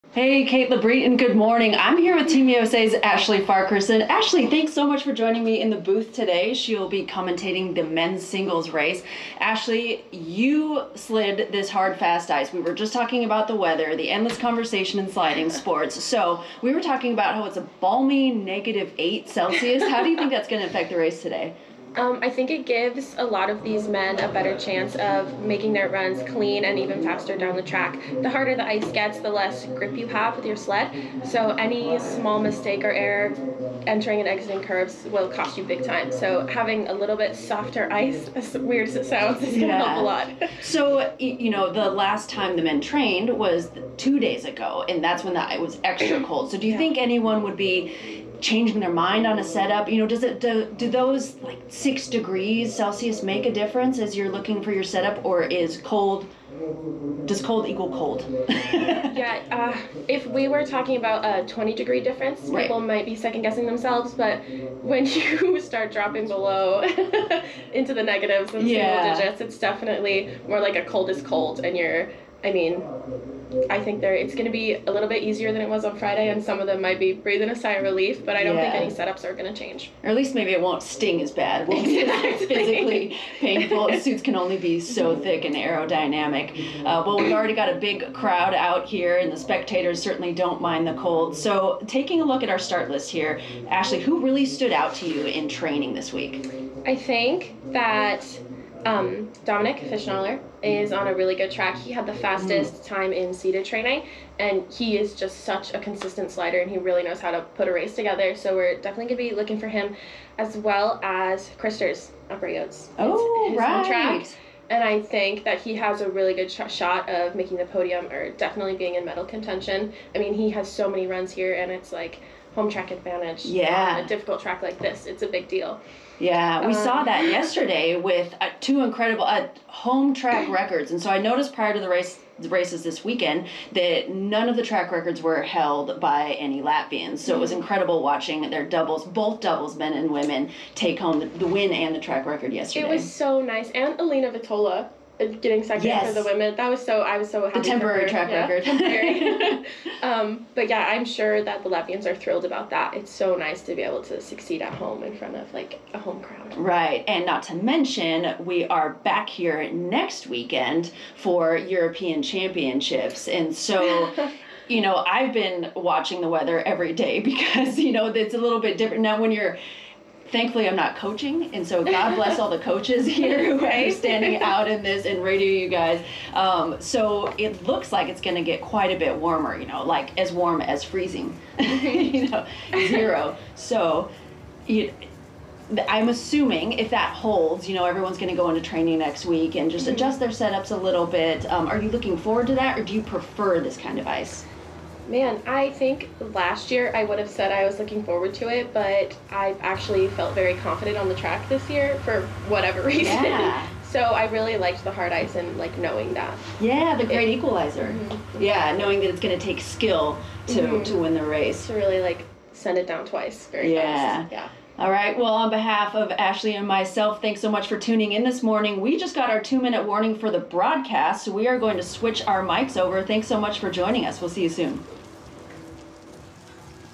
FIL Studio - Bree and Ashley review the Men's race
FIL Studio - Bree Schaff and Ashley Farquharson (USA) review the Men's race  here in Sigulda. @TeamUSA @USALuge